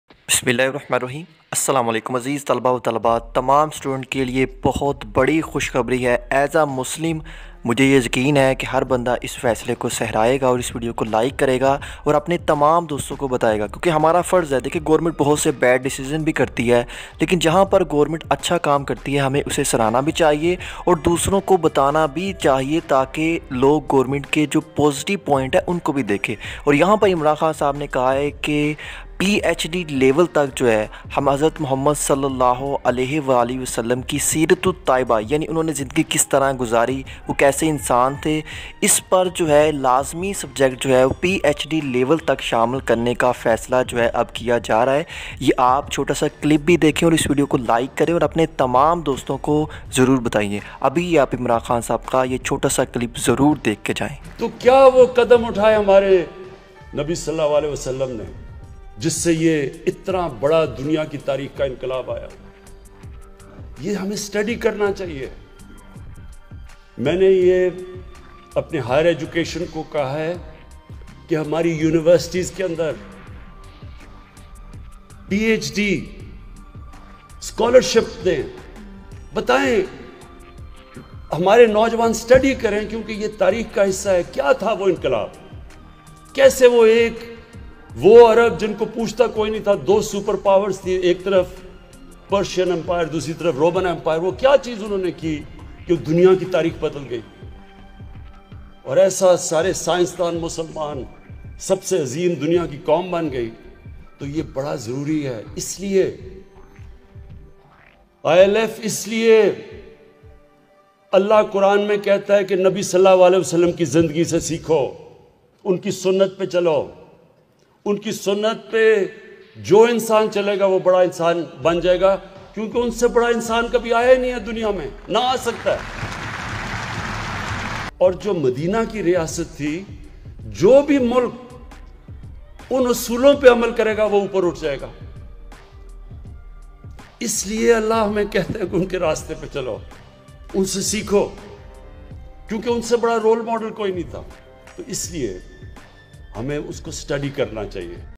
Good News For Students 2020 Imran Khan Great Decision Class 1 to PhD Students
In this video I tell you good news for every muslim students class 1 to PhD level great decision by imran Khan..